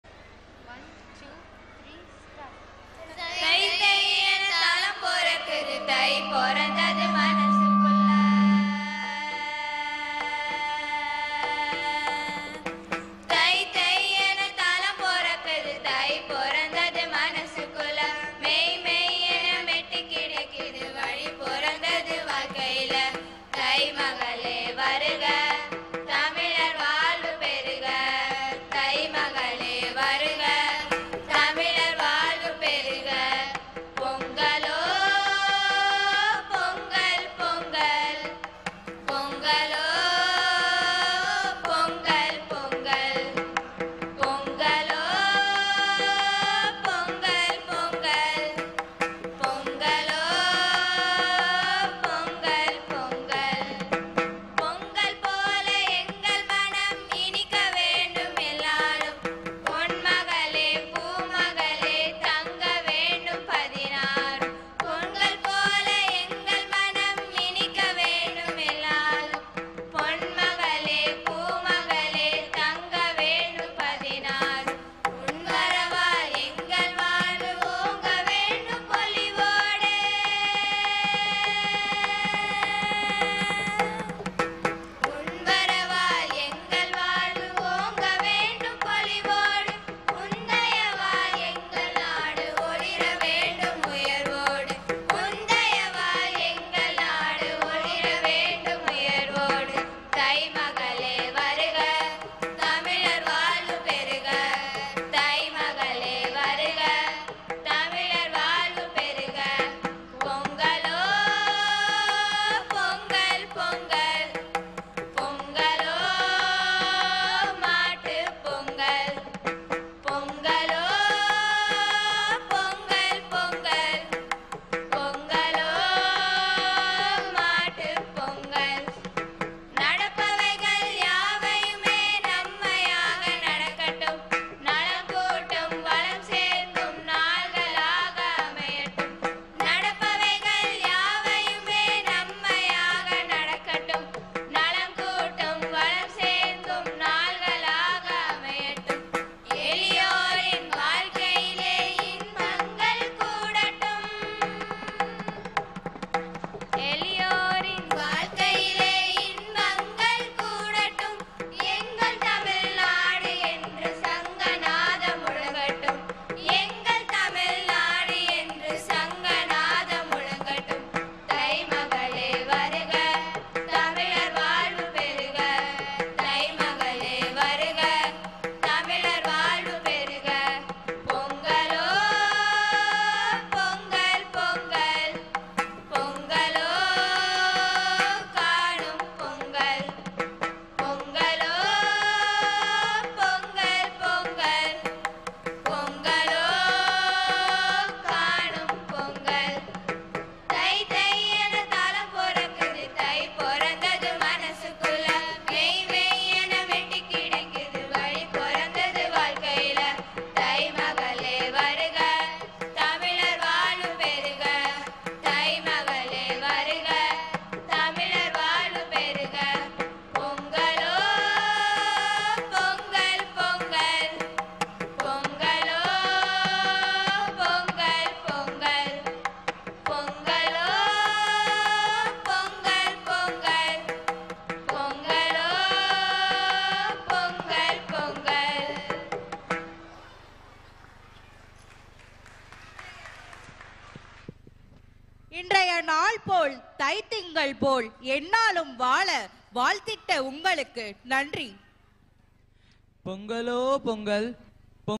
Pongal Songs Singing Best Performance|| Trinity Academy MHSS 33rd Annual Day School Function TRI V
Pongal Songs Singing Best Performance||  Trinity Academy MHSS 33rd Annual Day School Function  TRI VERBERATE 2024  Namakkal